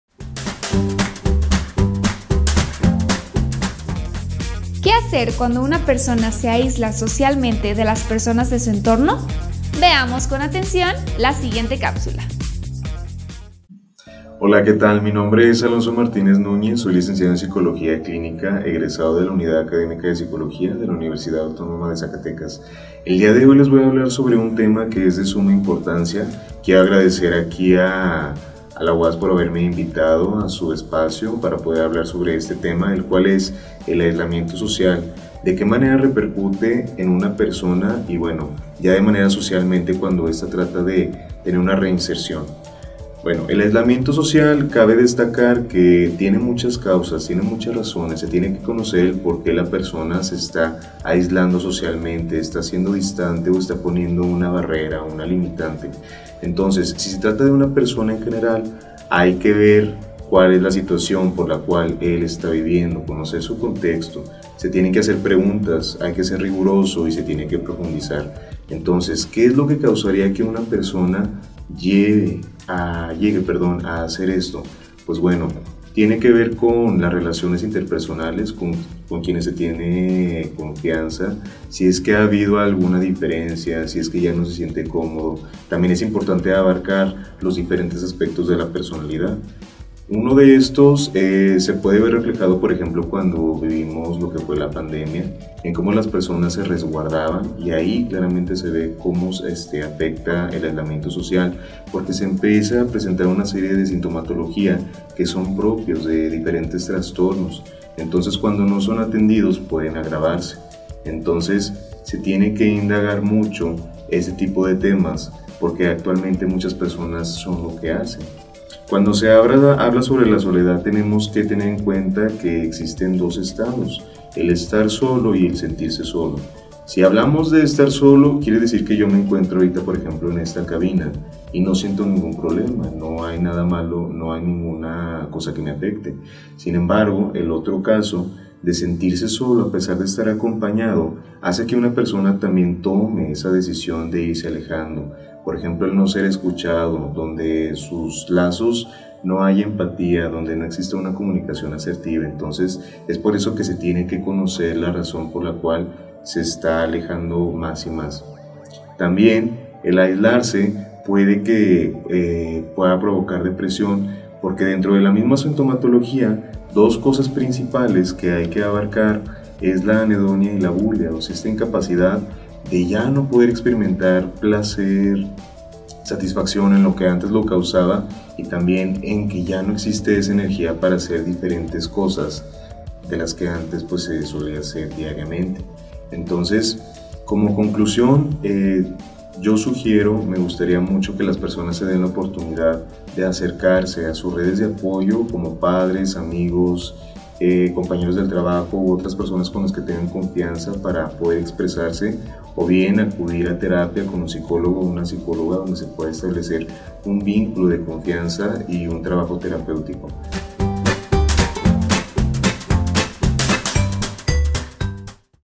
El aislamiento social. ¿Qué hacer para evitarlo?
¿Qué hacer cuando una persona se aísla socialmente? Alonso Martínez Núñez, egresado de la Licenciatura en Psicología de la UAZ, nos aconseja sobre este tema tan actual.